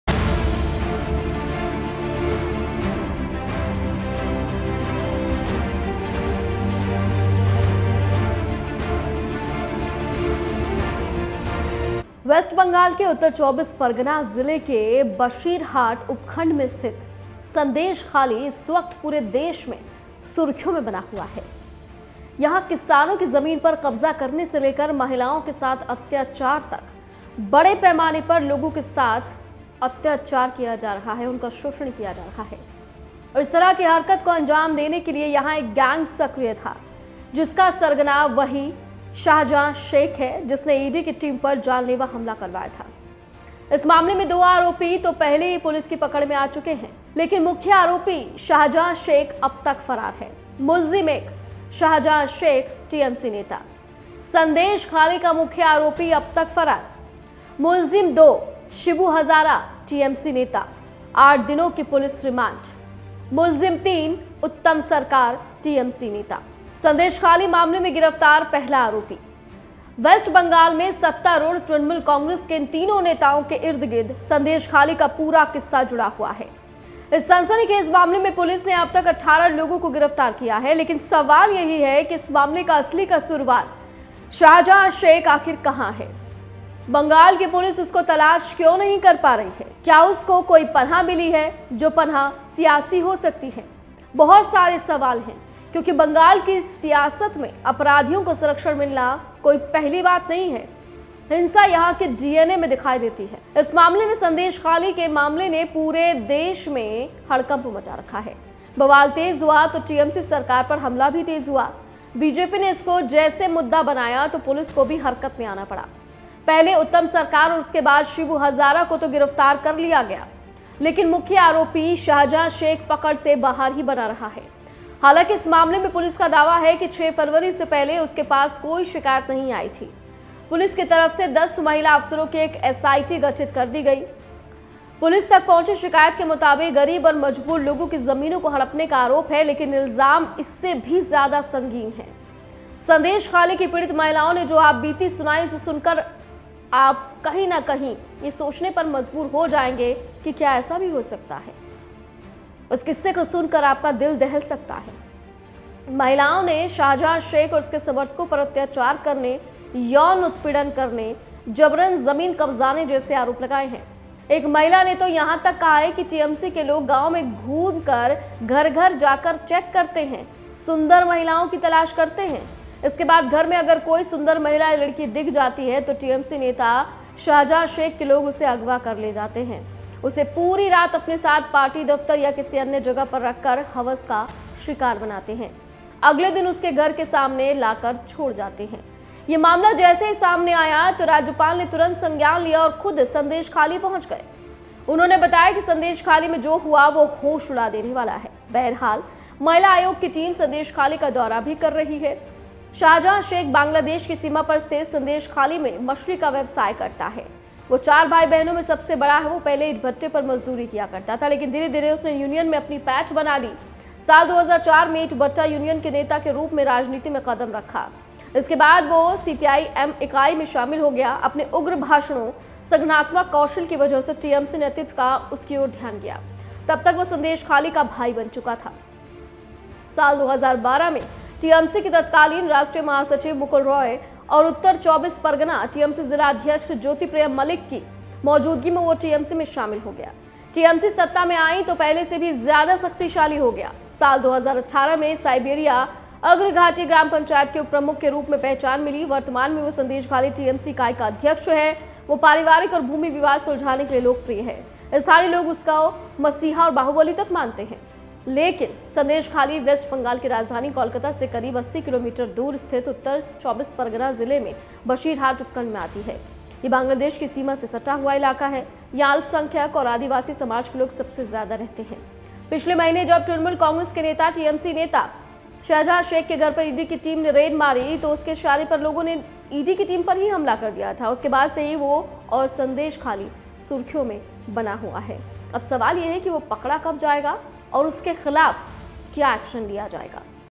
TMC के तीन नेताओं के इर्द-गिर्द संदेशखाली का पूरा किस्सा
वेस्ट बंगाल के उत्तर 24 परगना जिले के बशीरहाट उपखंड में स्थित संदेशखाली इस वक्त पूरे देश में सुर्खियों में हैं. किसानों की जमीन पर कब्जा करने से लेकर महिलाओं के साथ अत्याचार तक, यहां बड़े पैमाने पर लोगों के साथ अत्याचार किया गया है